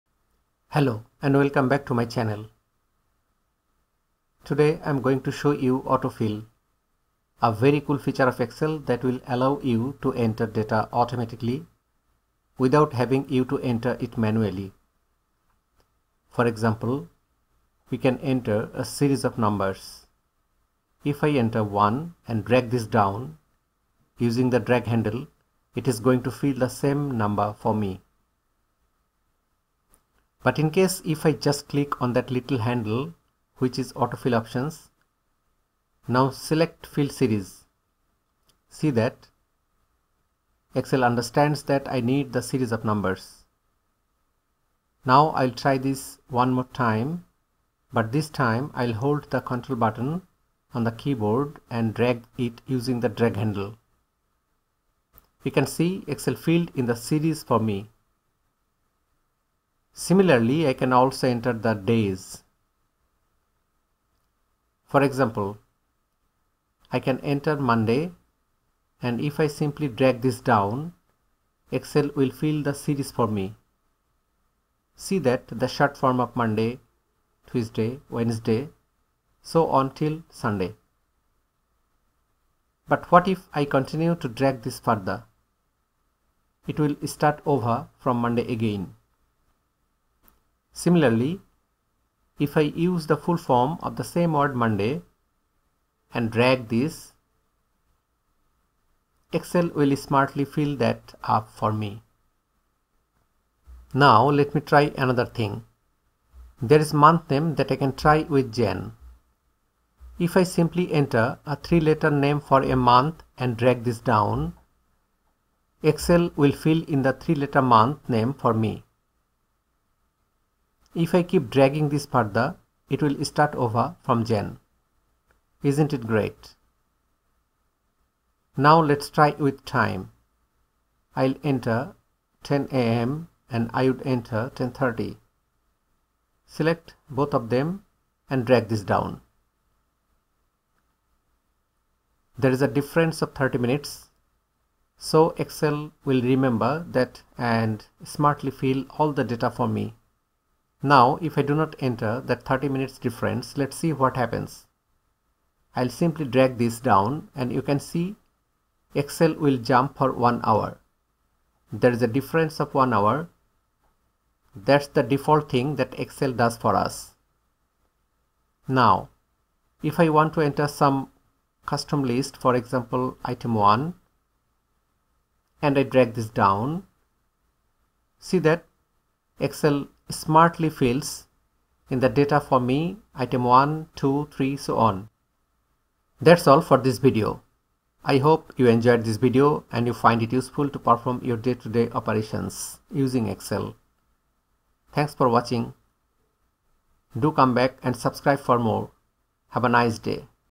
MS Excel Auto Fill | Excel Tutorial
This tutorial shows how to use Excel Auto Fill function with various examples.